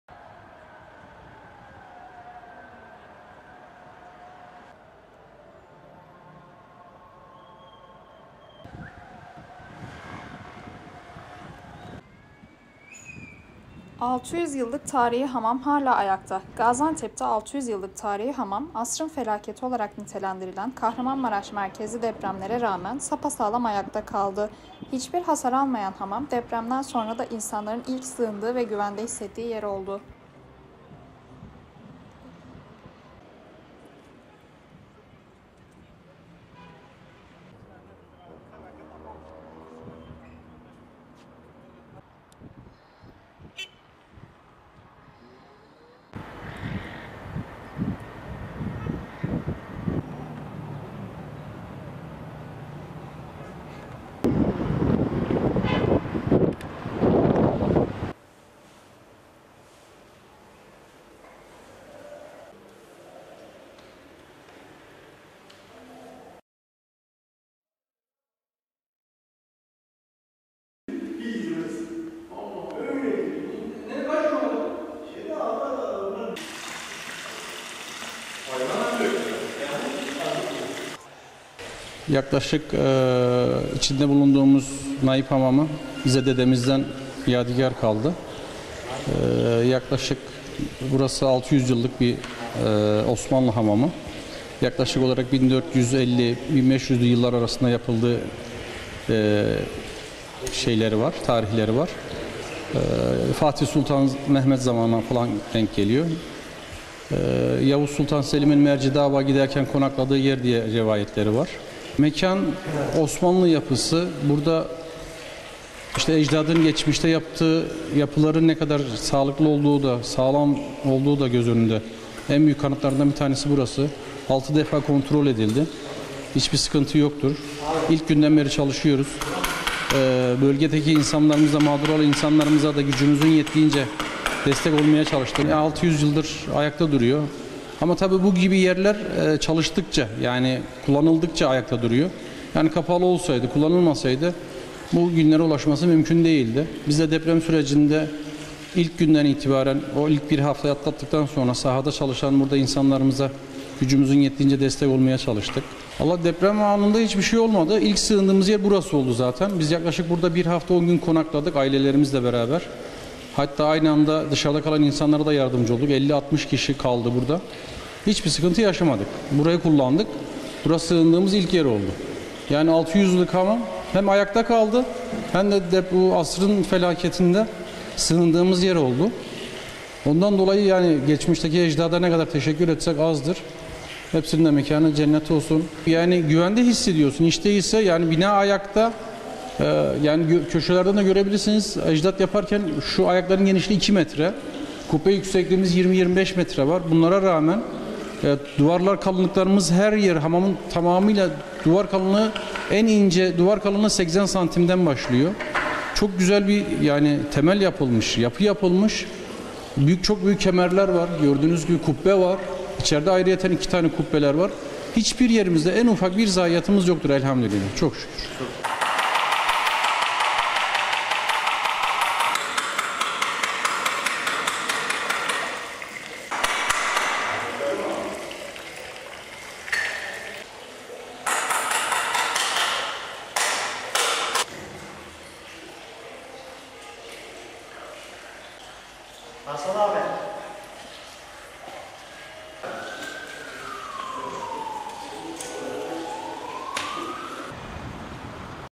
Gaziantep'te 600 yıllık tarihi hamam depreme meydan okudu
Gaziantep'te 600 yıllık tarihi hamam asrın felaketi olarak nitelendirilen Kahramanmaraş merkezli depremlere rağmen sapa sağlam ayakta kaldı. Hiçbir hasar almayan hamam, depremden sonra da insanların ilk sığındığı ve güvende hissettiği yer oldu.